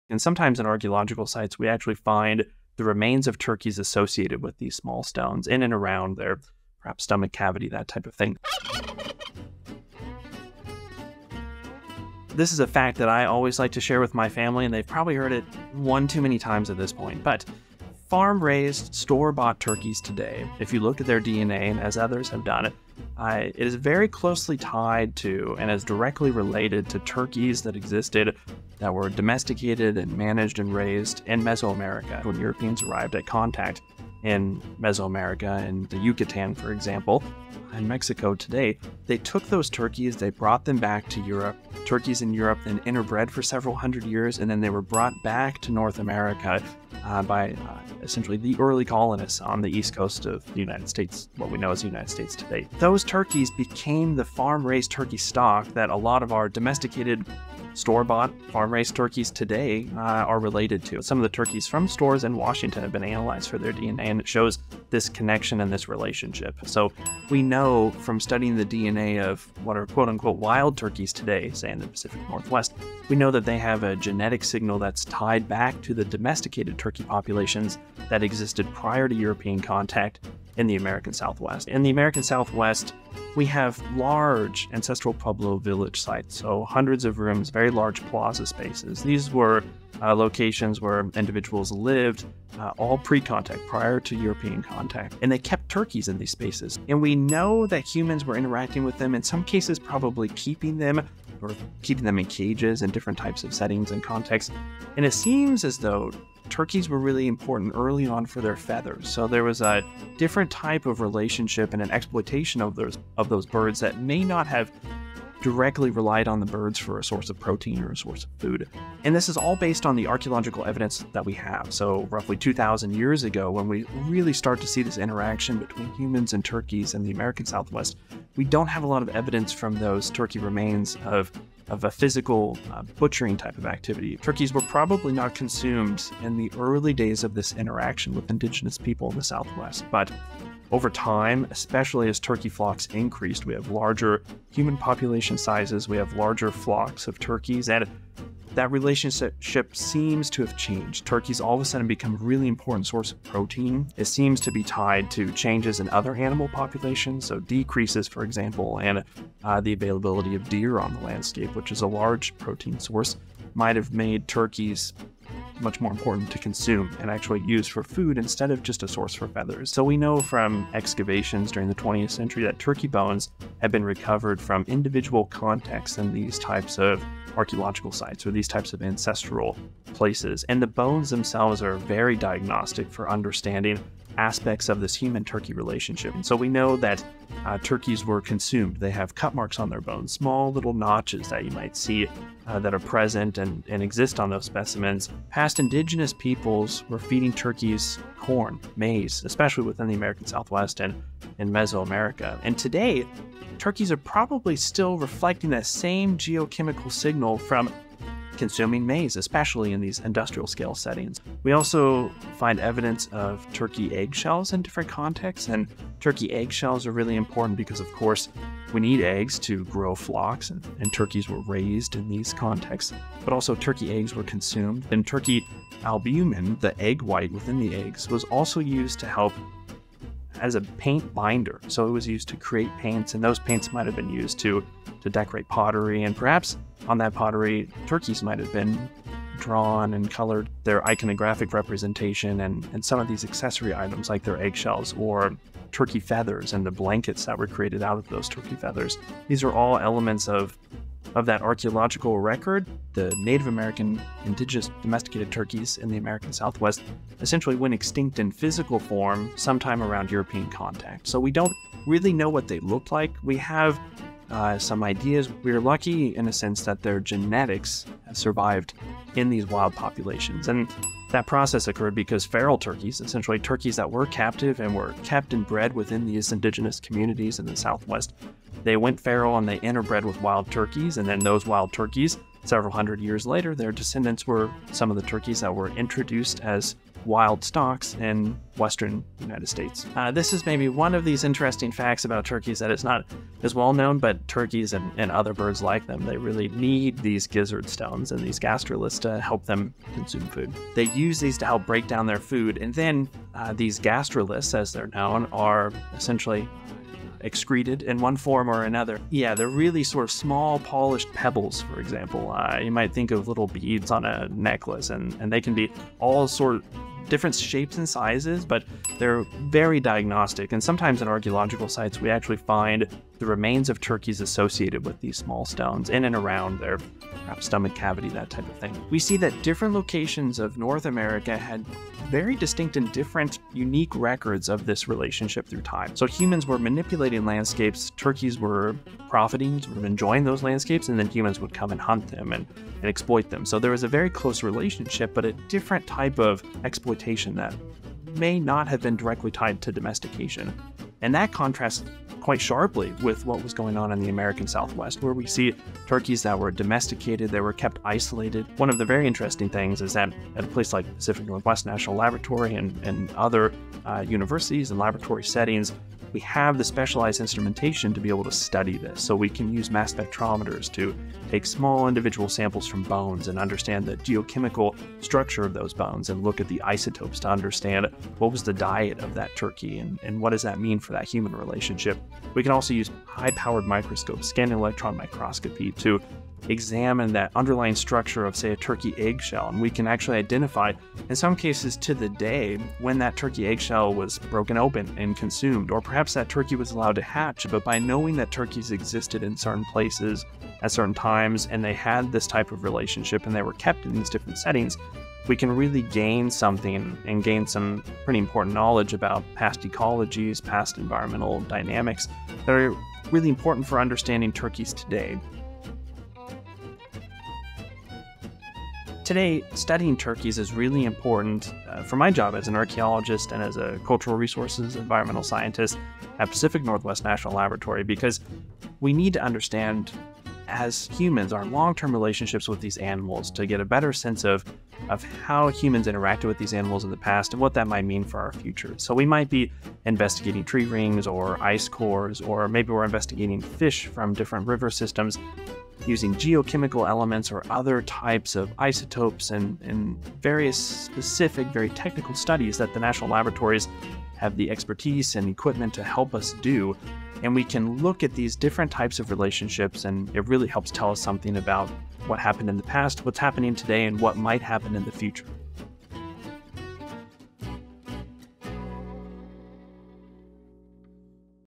33 Facts from a Turkey Expert
Before Zooarchaeologist Cyler Conrad came to PNNL to run environmental analyses for the Nuclear Regulatory Commission, his research began with the study of pre-contact human-turkey relationships. We asked him to share a few of his favorite facts about these majestic and complicated birds. Buckle in.